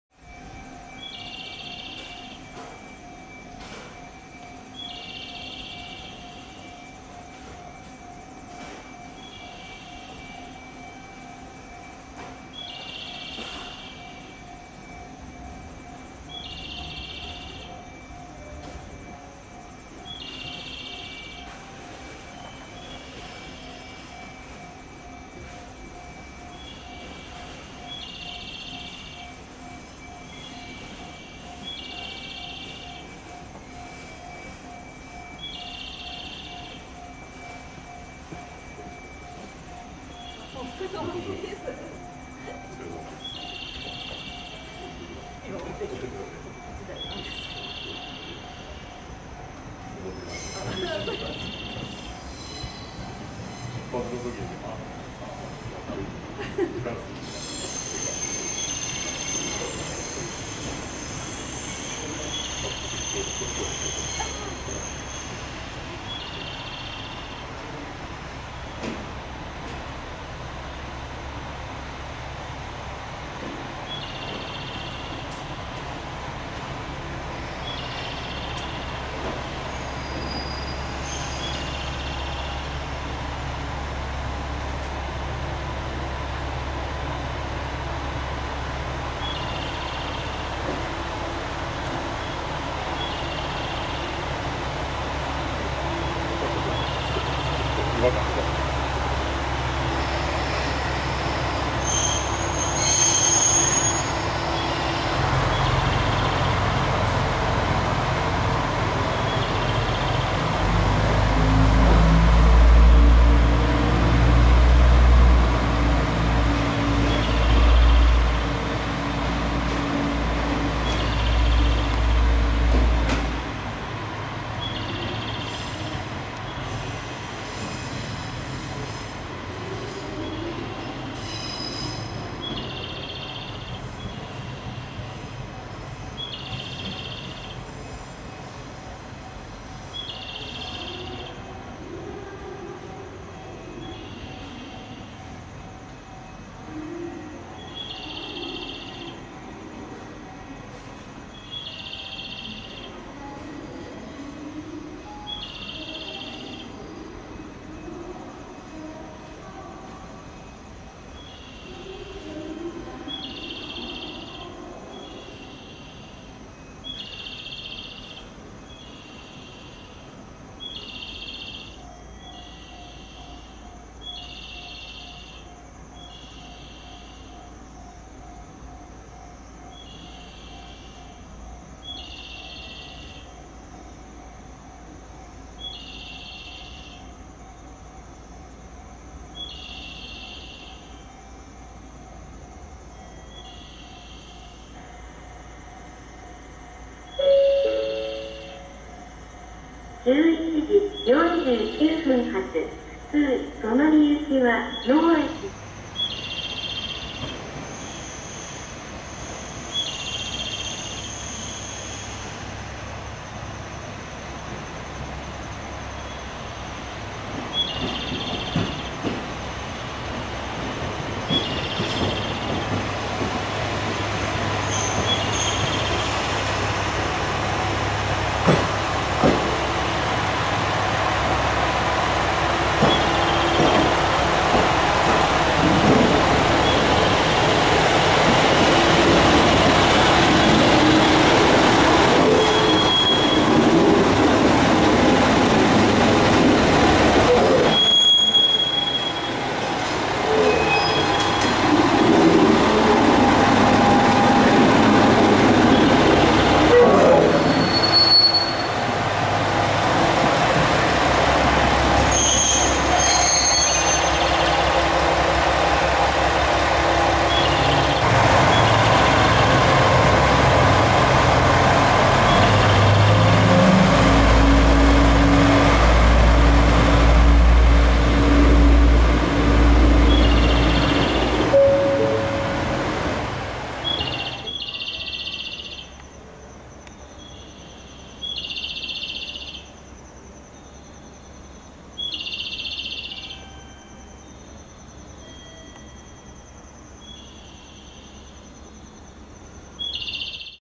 大糸線キハ120形・臨時サイクルトレイン 到着→留置線引き上げ
車種　JR西日本キハ120形
日時　2022/09/21
場所　糸魚川駅
撮影機材　SONY AX-60